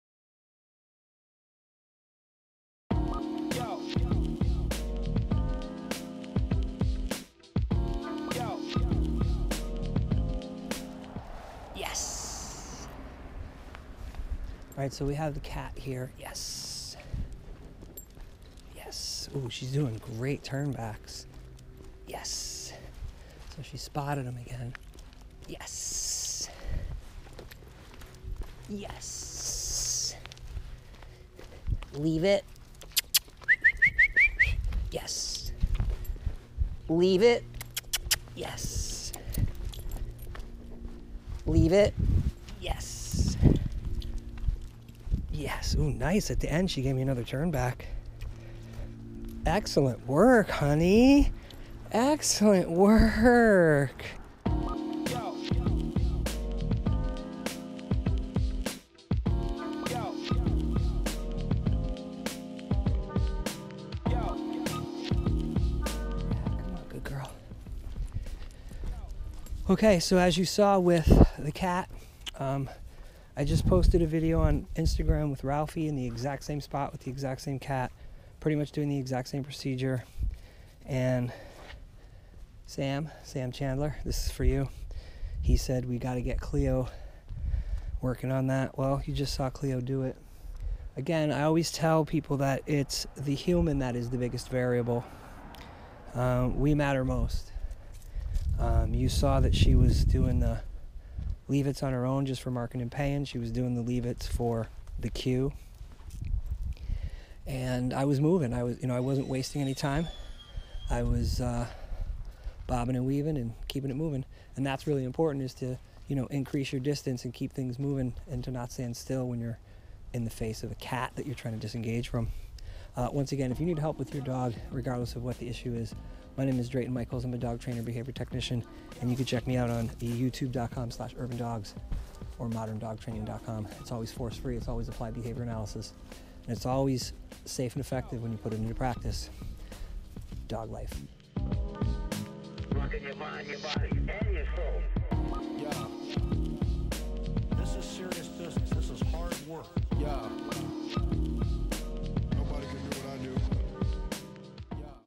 CLEO DISENGAGE FROM CAT
Disengagement from prey is difficult but not impossible. What is needed is practice counter conditioning, using the leave it cue, and when needed issuing removal - time outs for lunging out.
Cleo is a 2 year Pit Bull terrier that has a high prey drive but has been worked with extensively to achieve the great results seen in this video! Enjoy!